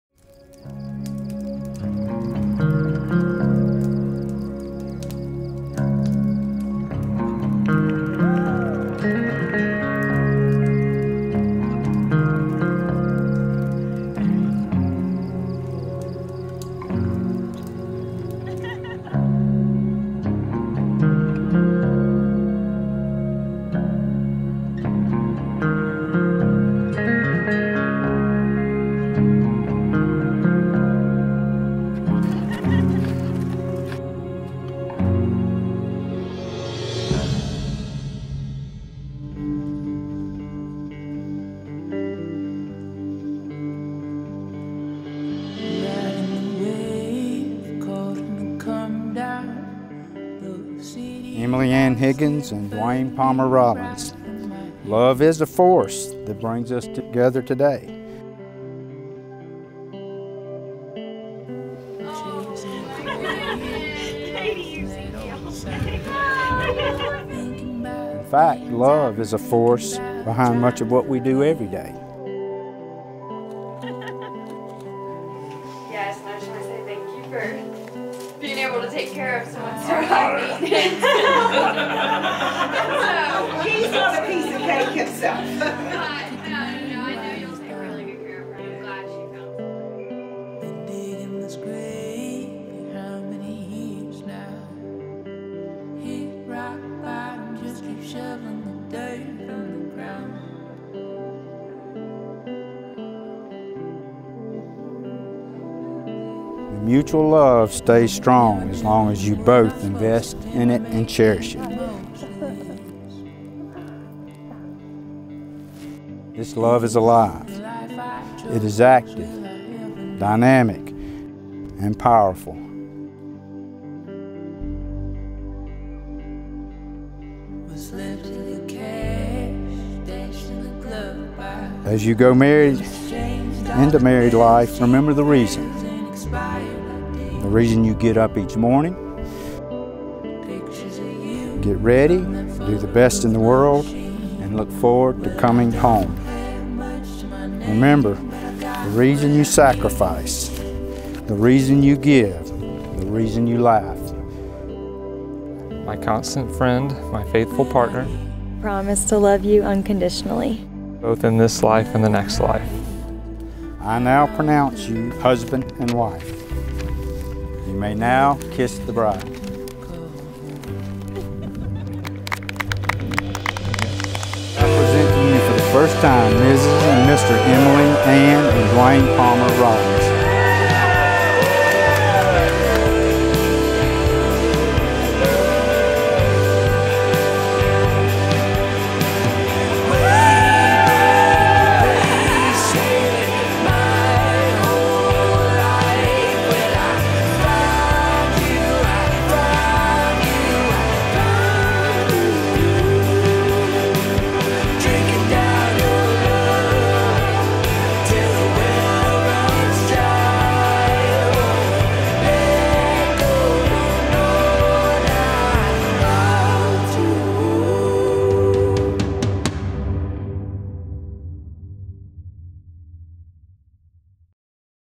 Intimate Elopement at the Cactus Moon Retreat in Joshua Tree, CA | Emily & Dwayne
Emily and Dwayne celebrated their love at the Cactus Moon Retreat just outside one of southern California's most gorgeous national parks, Joshua Tree. We spent the entire day with them capturing all of the intimate moments happening throughout the day and wandered the national park with our friend Amber Moon Photo for such a fun and beautiful romantic's session. Their ceremony brought us to tears with what was said and the view of the San Jacinto mountains was a sight to see. We ended the evening with some impromptu toasts and a bonfire where Emily and Dwayne took a minute to just relive their day. There is something so special about intimate elopements that we love. We cannot wait to do it again!

Venue: @cactusmoonretreat
Photographer: @Ambermoonphoto

Checkout more of our work here!

Inquire Here!